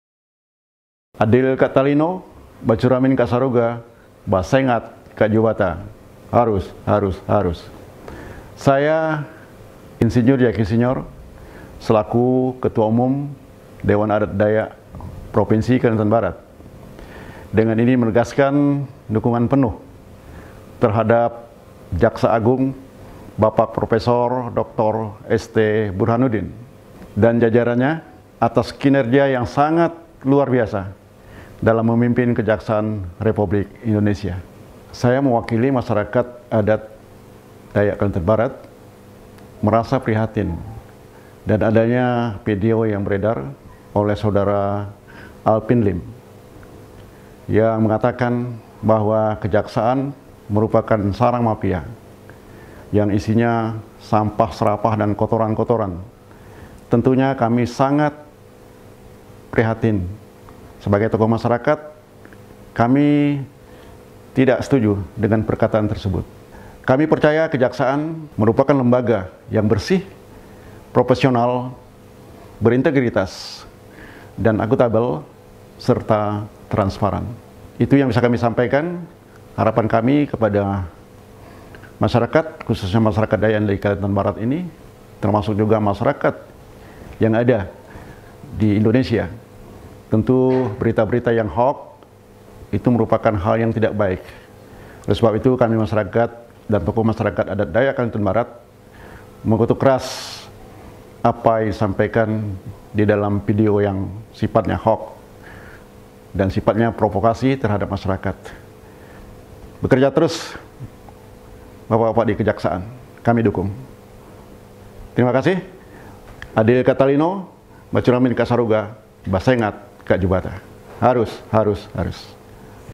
Dewan Adat Dayak (DAD) Kalbar, Mengecam Pemberitaan Hoax Tentang Kejaksaan RI
‎@Kejati Kalbar Penkum